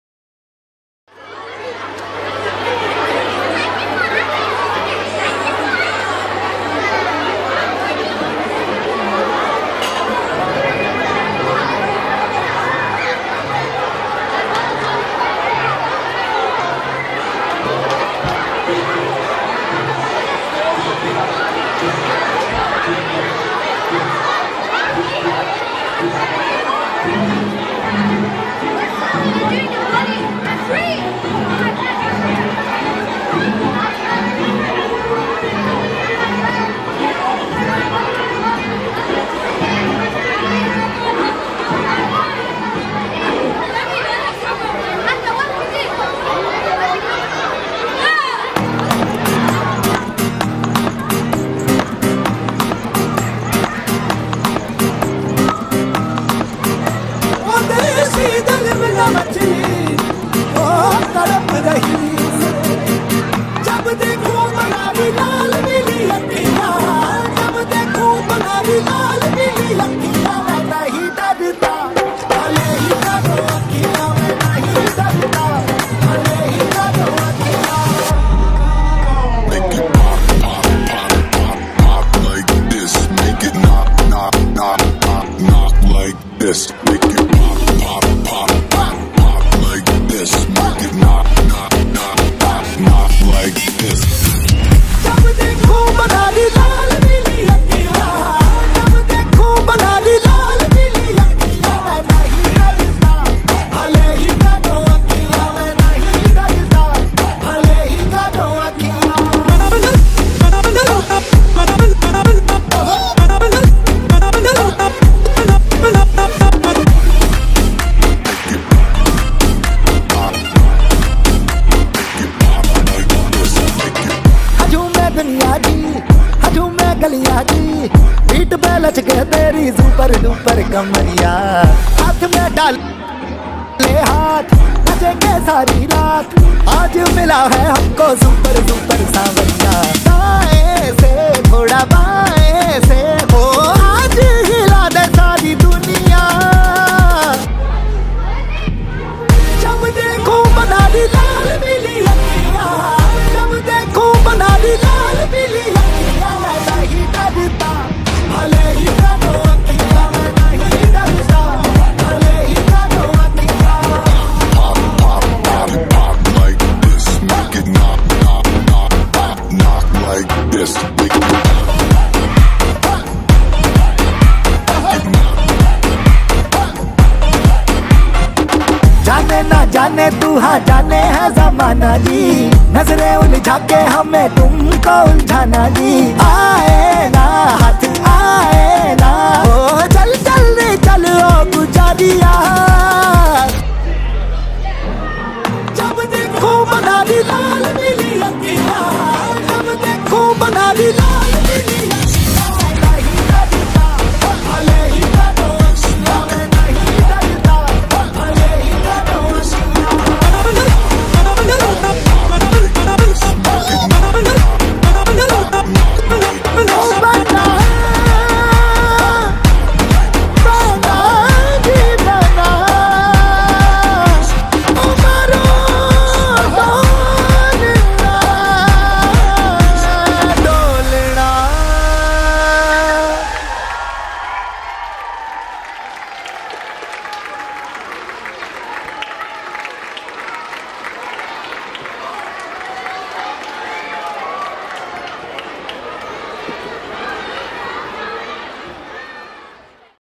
Year 5 Eid concert flash mob 2024
Year 5 performing their bollywood dance in the playround after school!